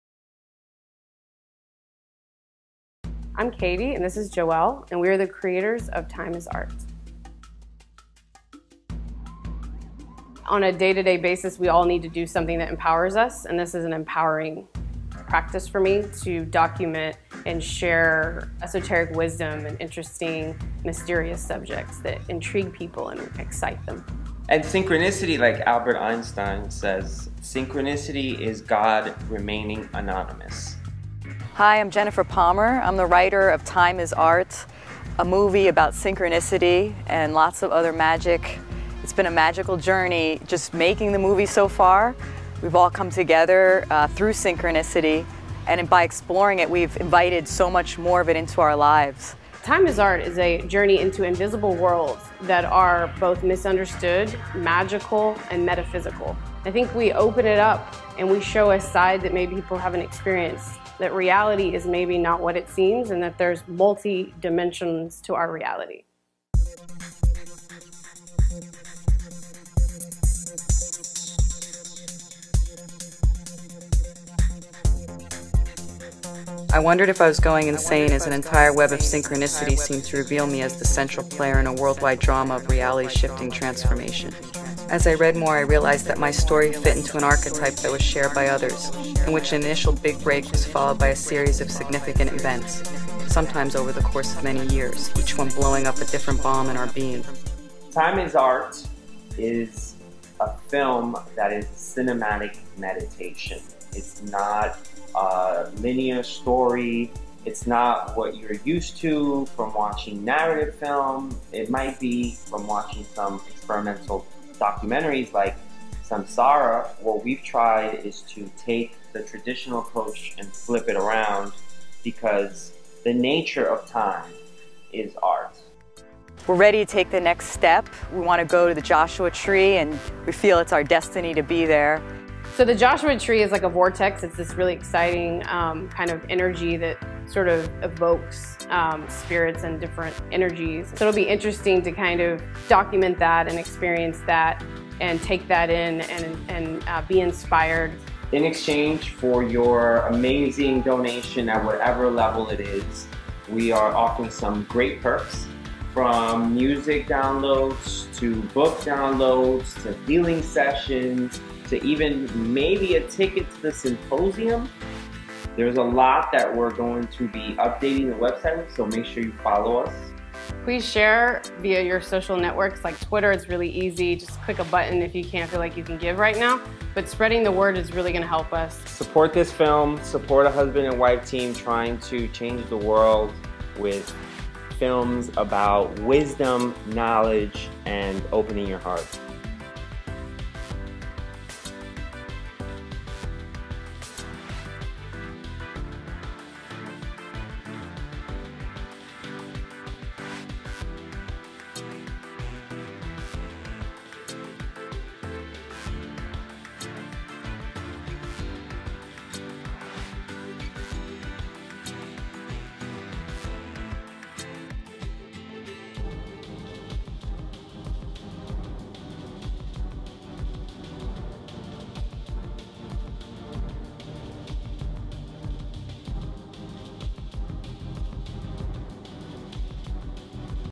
Support Time is Art on Indiegogo
Thanks for helping us raise over $5k to make our trip to Joshua Tree successful! We filmed Richard Tarnas, Toko-pa Turner, Rupert Sheldrake, Graham Hancock and many other inspiring people.

Time is Art: Synchronicity & the Collective Dream is a artful meditation on the nature of reality, time and meaningful coincidence. Helping to see with your heart what was once invisible.